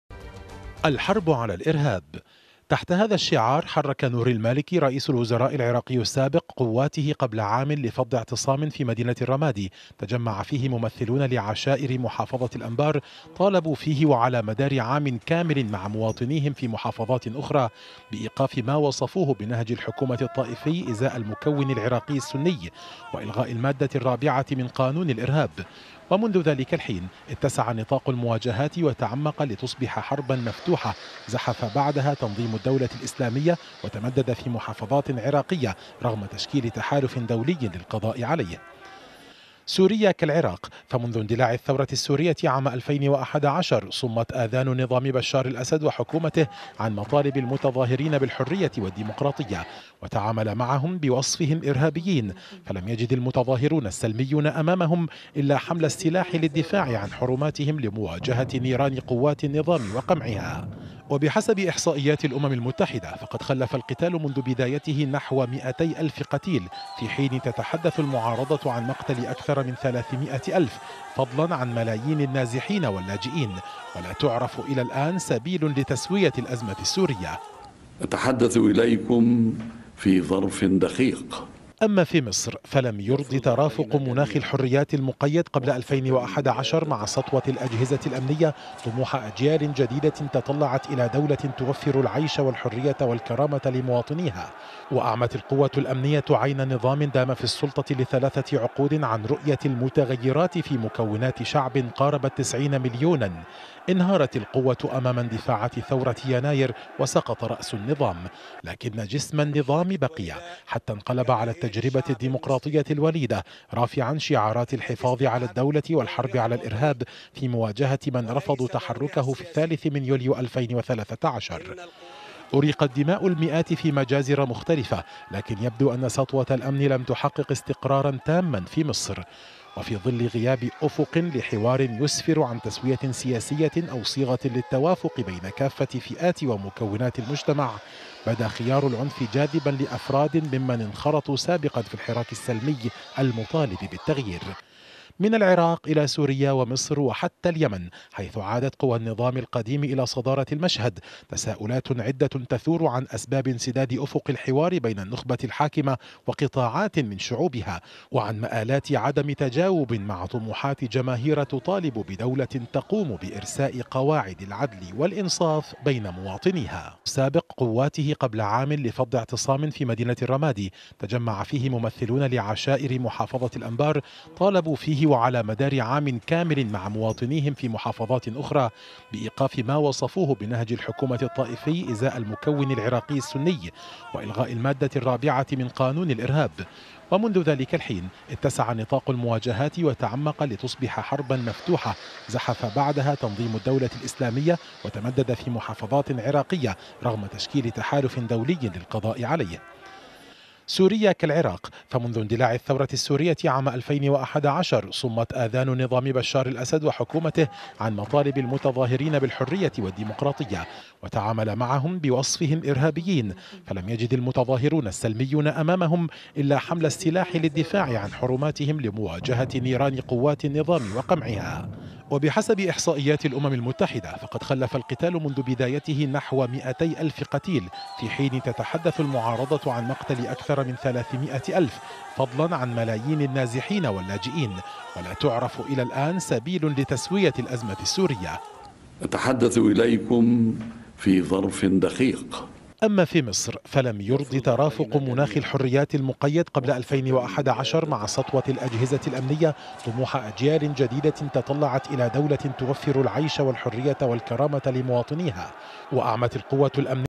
&x202b;سبل تحصين المجتمعات العربية في مواجهة التطرف و
في أيام كهذه قبل عام، اندلعت مواجهات مسلحة في محافظة الأنبار العراقية بعدما تدخلت القوات الحكومية لفض اعتصام احتجاجي في مدينة الرمادي، طالب المشاركون فيه على مدى عام كامل بإصلاحات سياسية وقانونية.